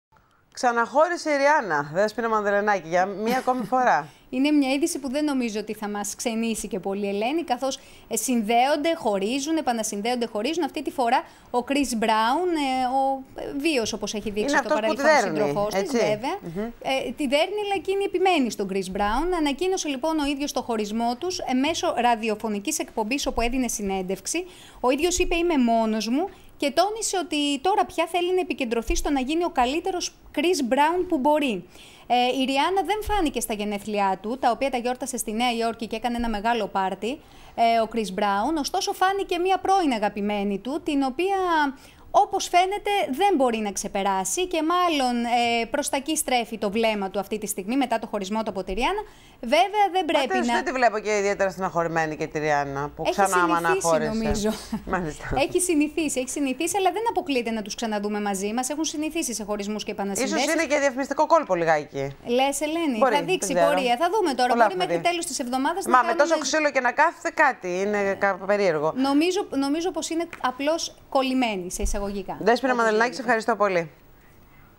8.5.13-Ο Κρις Μπράουν ανακοίνωσε από το ραδιόφωνο το χωρισμό του με τη Ριάννα
Μεσημεριανό δελτίο ειδήσεων, Τετάρτη 8.5.2013, με την Ελένη Τσαγκά.
Ο Κρις Μπράουν ανακοίνωσε από το ραδιόφωνο το χωρισμό του με τη Ριάννα